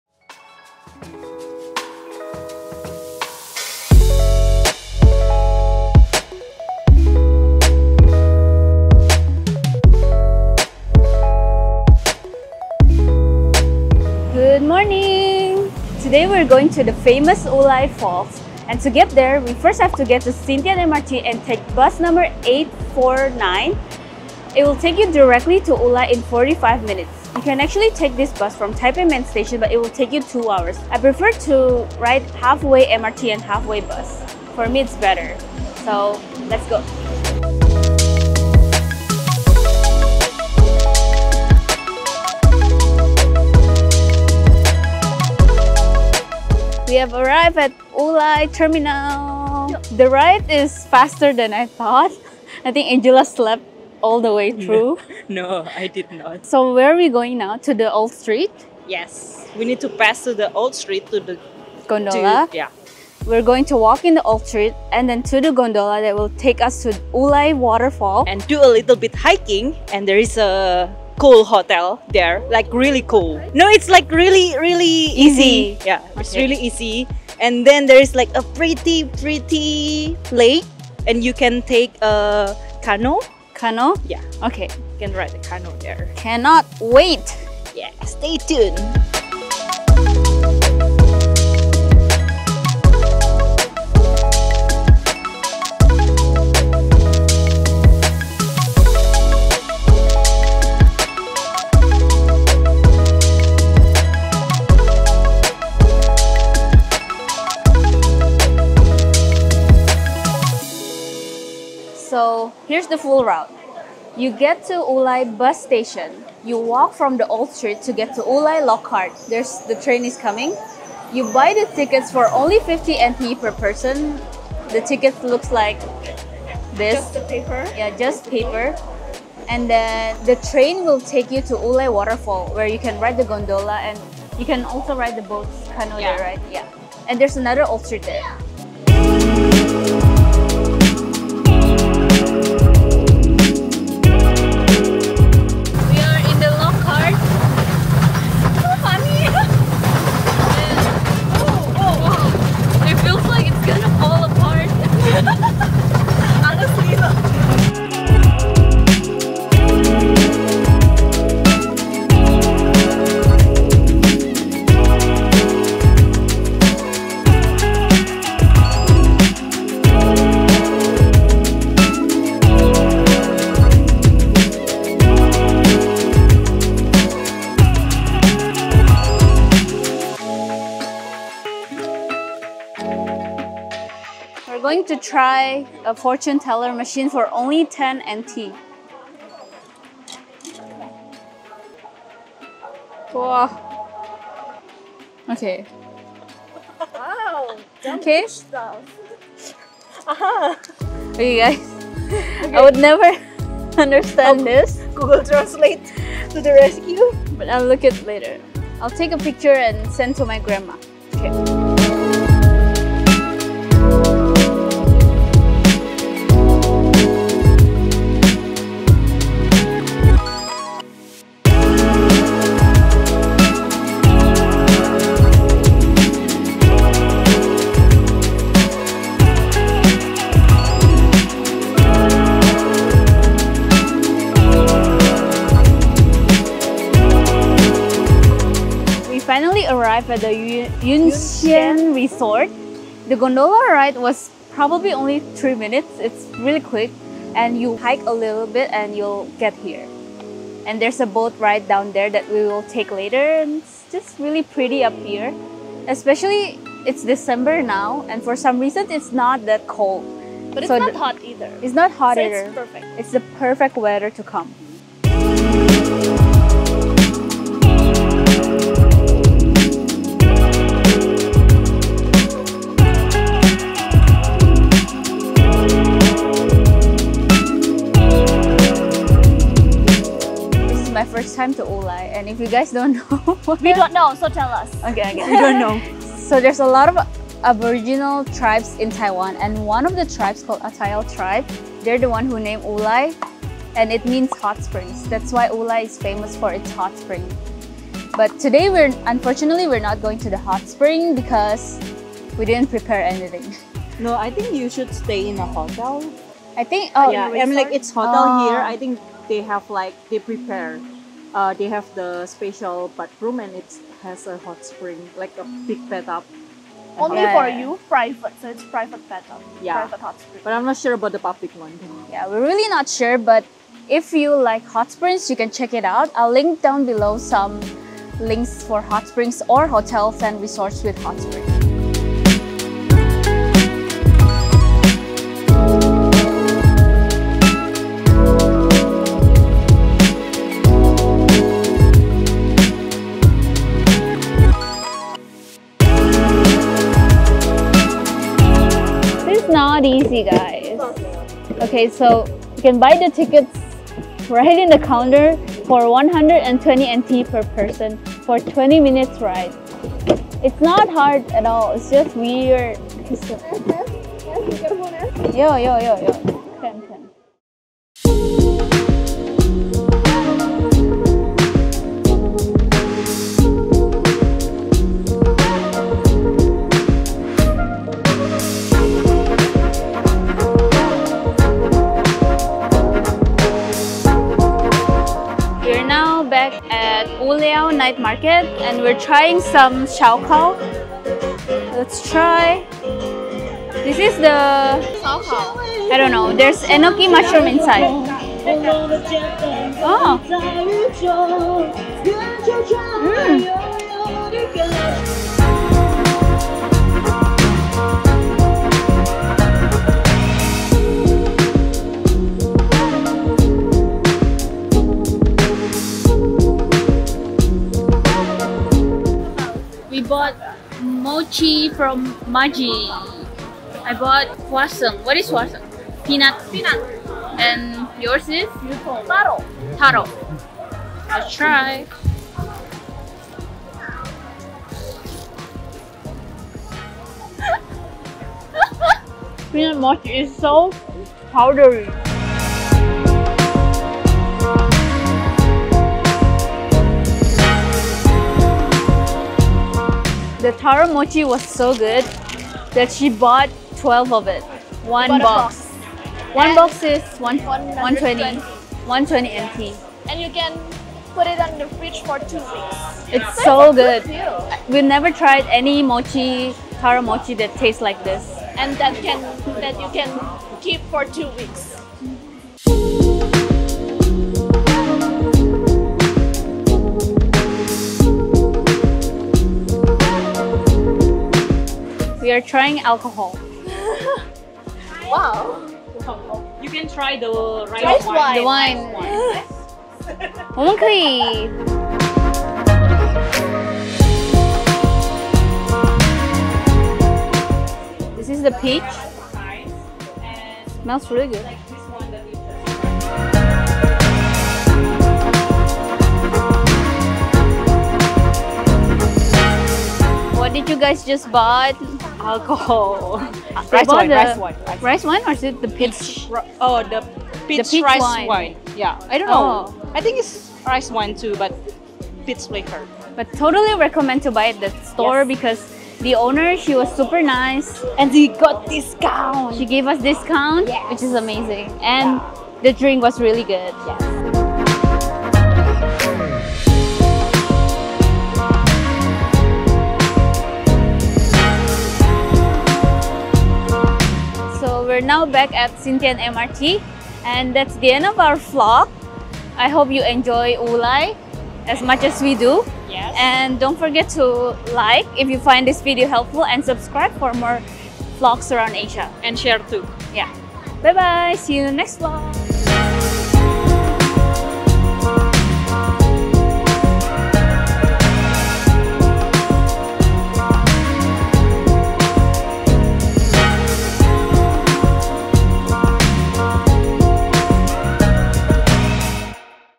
Day trip TAIPEI to WULAI Waterfall & Old Street | MUST VISIT IN TAIWAN 🇹🇼
One popular destination just an hour away from Taipei City is Wulai. In this vlog, you’ll learn the complete route to get to Wulai, including the charming log card and gondola ride to Yun Hsien Resort for a boat ride. Additionally, you’ll get a glimpse of what to eat and buy at Wulai Old Street.

0:00 - 0:46 - How to Get to Wulai from Taipei
0:47 - 1:46 - Arrive to Wulai Terminal Station
1:47 - 3:04 - Ride to Wulai Falls & Gondola to Yun Hsien Resort
3:05 -4:24  - Arrive at Yun Hsien Resort
4:25 - 6:07 - Attractions at Yun Hsien Resort
6:08 - 8:54 - Wulai Old Street
8:55 - 9:39 - Back to the city

👇🏻 Hotel &  Attractions in Wulai
[HOT SPRING HOTELS]

Links to support our travel journey to keep going 🫶🏻

(You can redeem it now and use it anytime you’re shopping with KLOOK.)